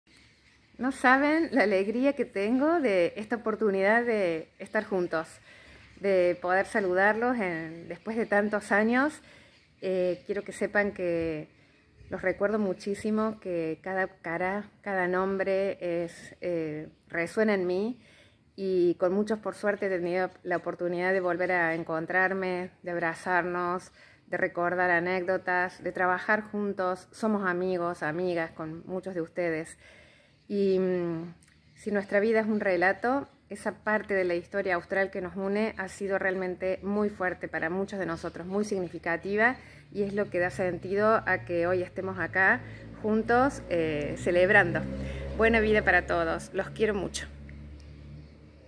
Marcela Farré nos envía sus saludos
Con ocasión de nuestro 30 aniversario, la profesora Marcela Farré, gran amigo de la casa, nos manda sus felicitaciones.